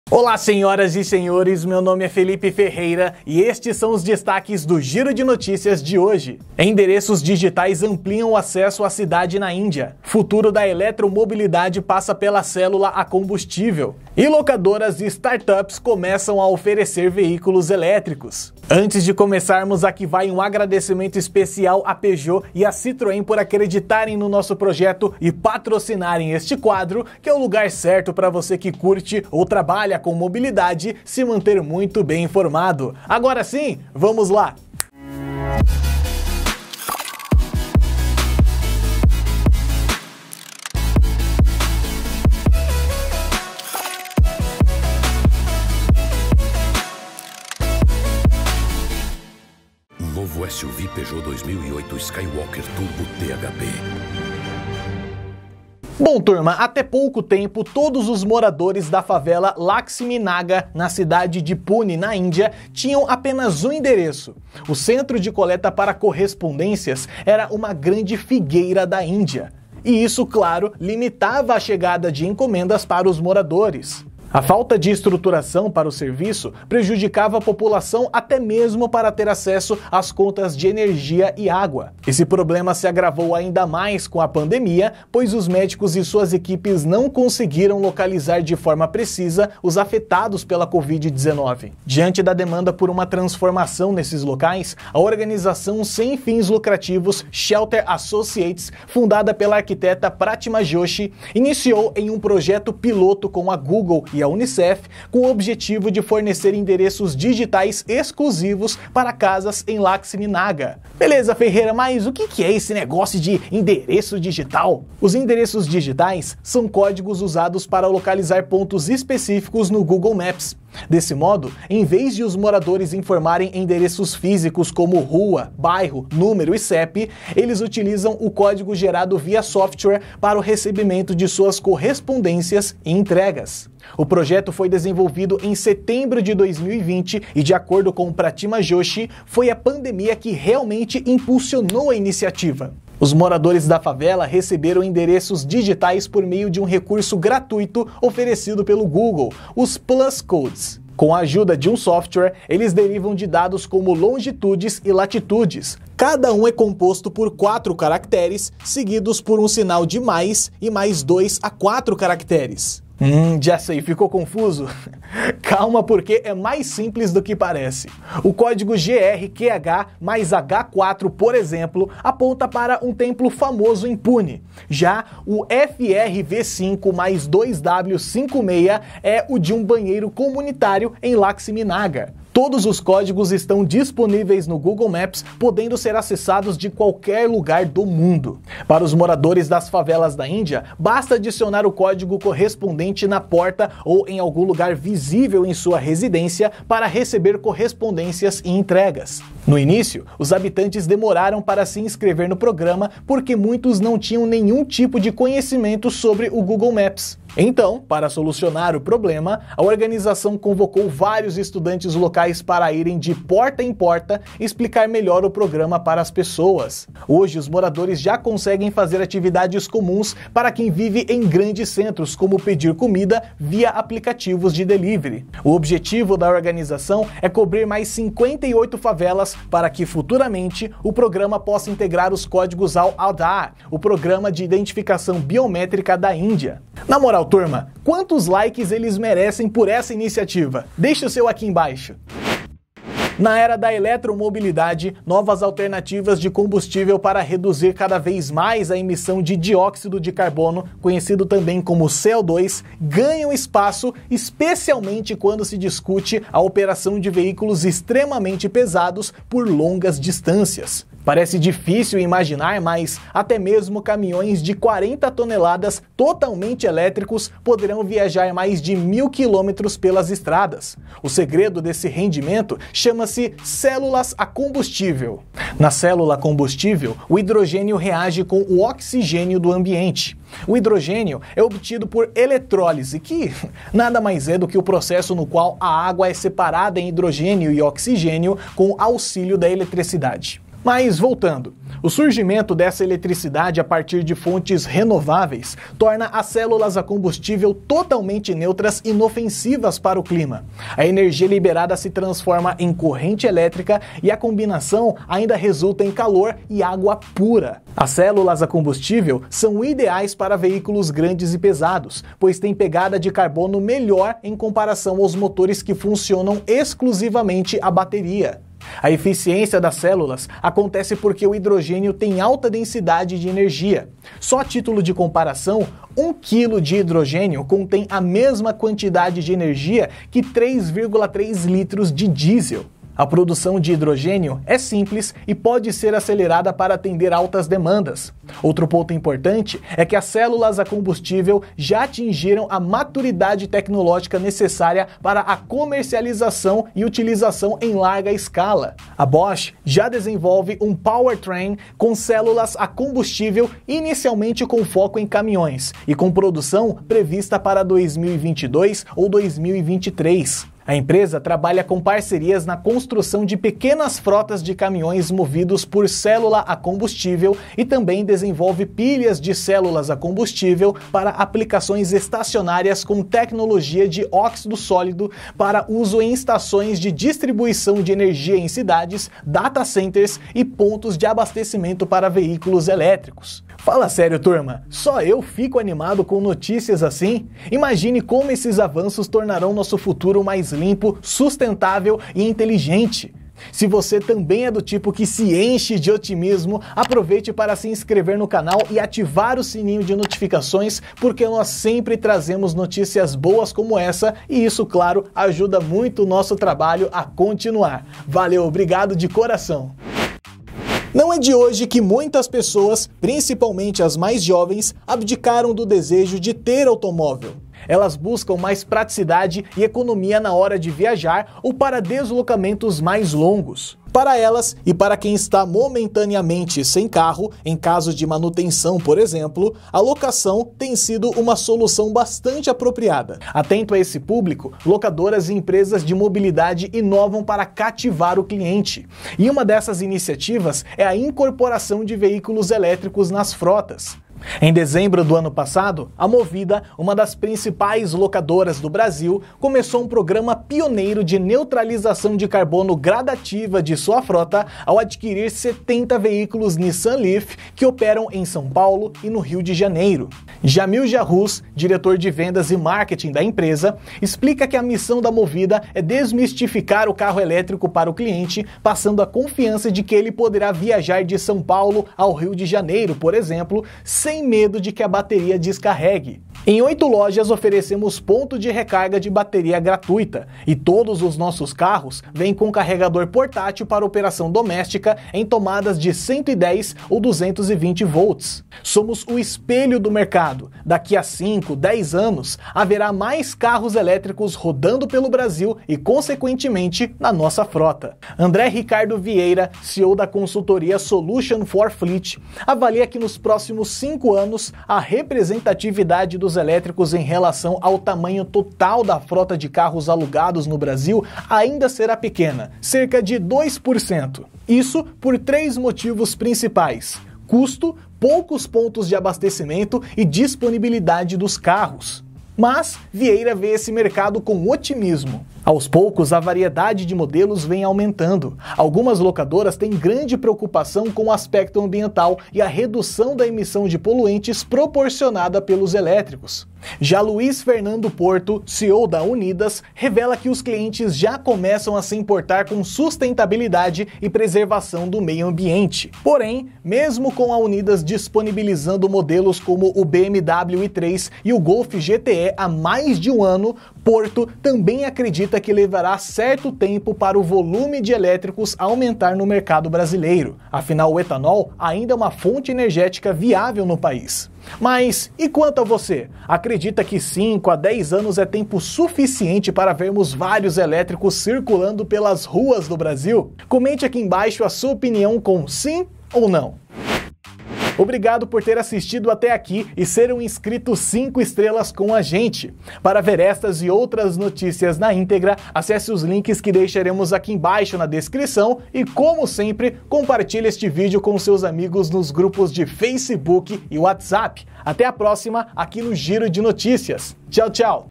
GIRO DE NOTÍCIAS #26 | PROGRAMA MOBILIDADE
Elétricos chegam às locadoras; endereços digitais nas favelas e células a combustível para pesados

Para conferir todos os conteúdos na íntegra, acesse:

Endereços digitais ampliam o acesso à cidade na Índia